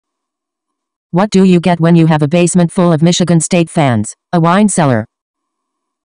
Jokes - What do you get when you have a basement full of Michigan State fans? A whine cellar.
Joke :
What do you get when you have a basement full of Michigan State fans?
A whine cellar.